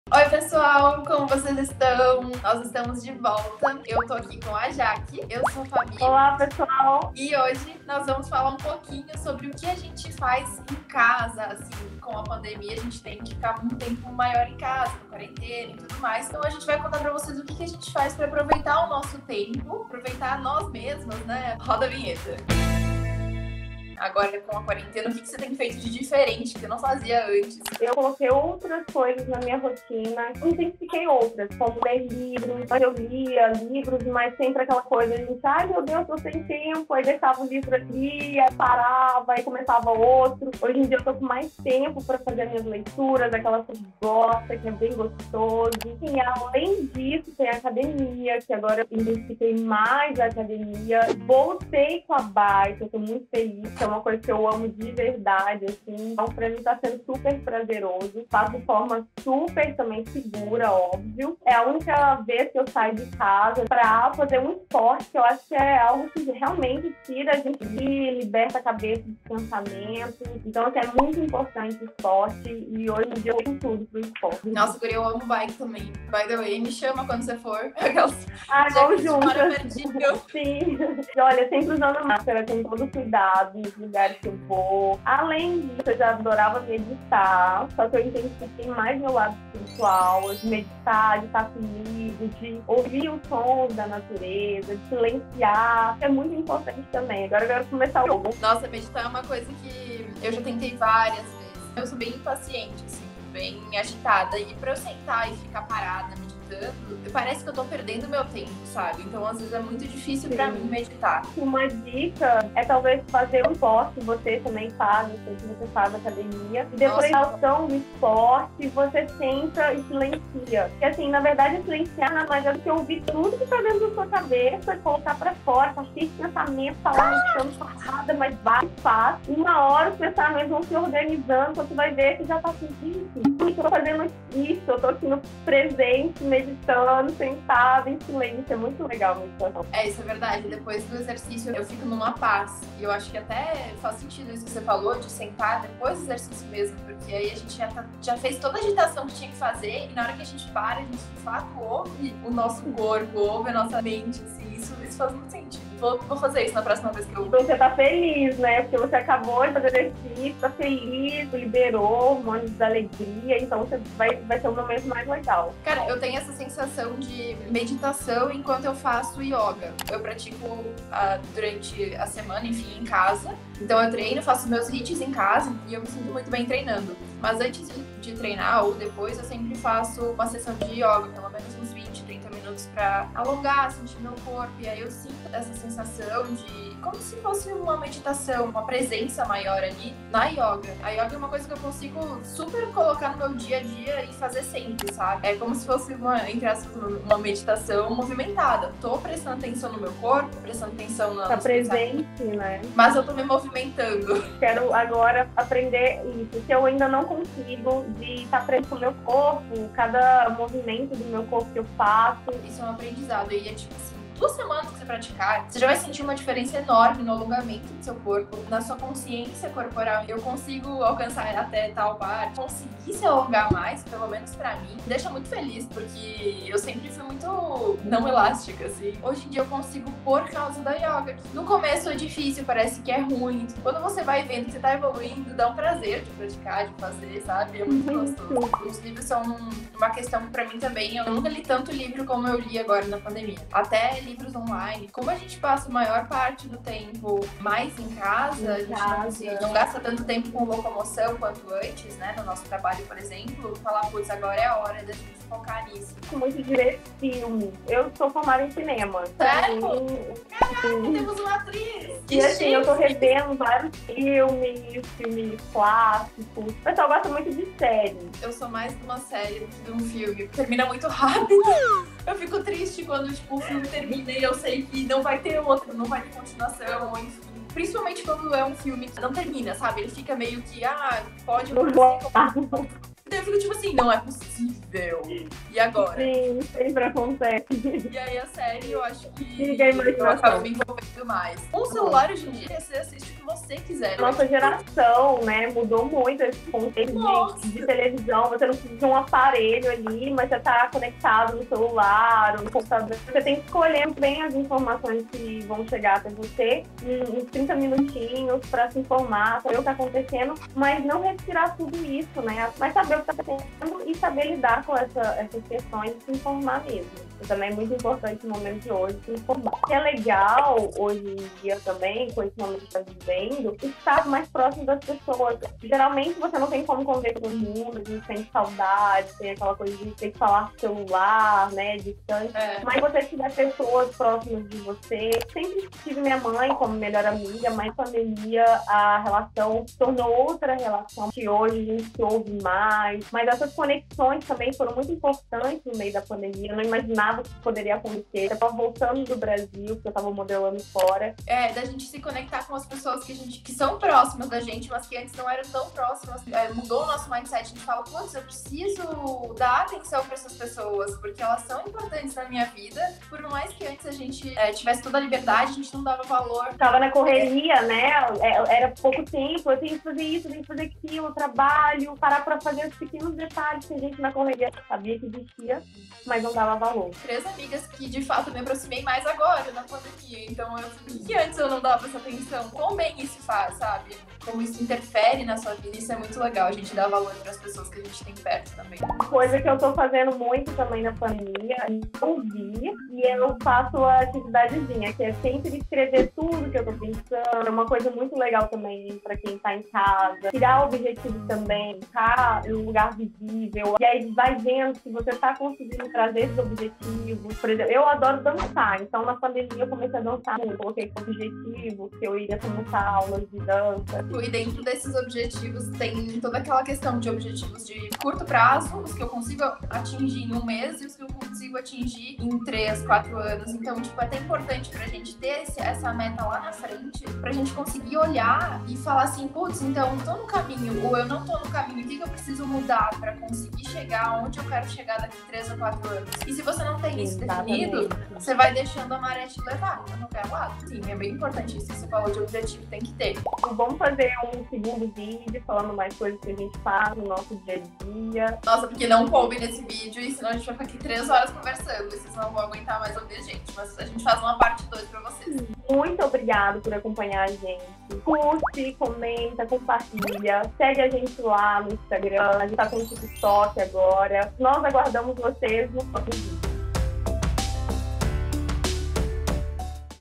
AUTOCUIDADO: COMO APROVEITAMOS NOSSA COMPANHIA E ESPAÇO | ESPELHO MEU
Oi, meninas. Tudo bem com vocês?
Vamos bater um papo sobre autocuidado, o que gostamos de fazer no nosso tempo livre para aproveitar nosso espaço e companhia, utilizando leitura, meditação e exercícios físicos pra nos desenvolver e nos tornarmos mais saudáveis.

Não esquece de dar um like no vídeo e comentar o que vocês gostam de fazer!